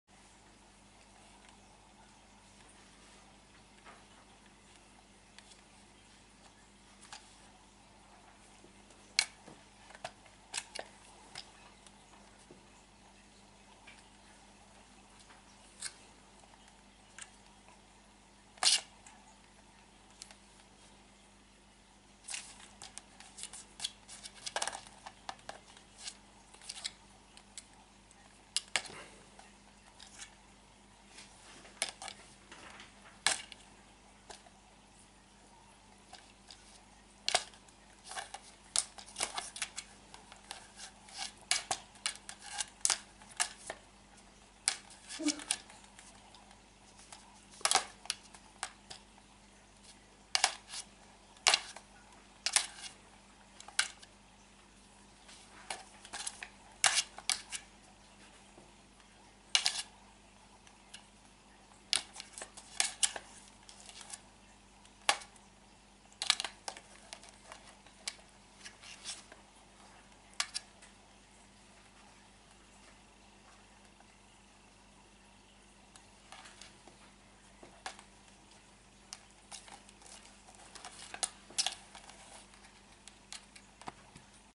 Star Wars Jedi Kater gegen Laserschwert
Unser Kater könnte bei Star Wars als Jedi gegen jeder mit einem Laserschwert antreten.